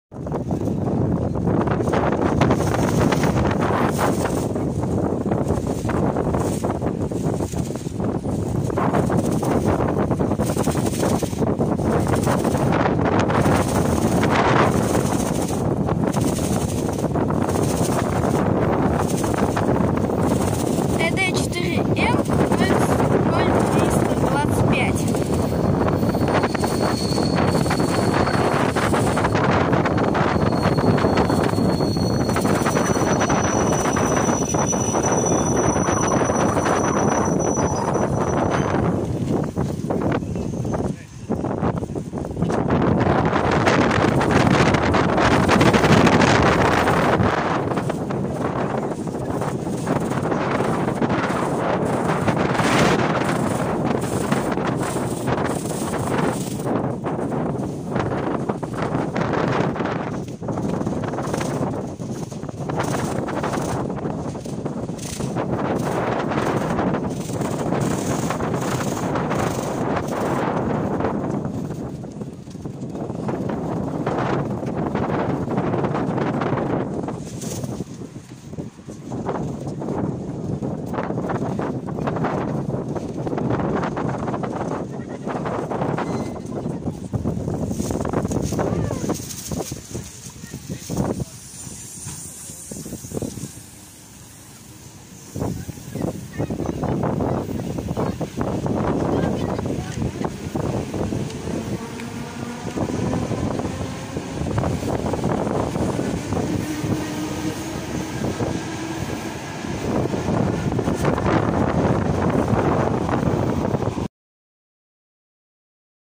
РУССКИЙ ВЕТЕРОЧЕК! Электропоезд ЭД4М 0325 с маршрутом 6918 Москва Курский вокзал-Петушки.
Пригородный электропоезд с маршрутом 6918 Москва Курский вокзал-Петушки.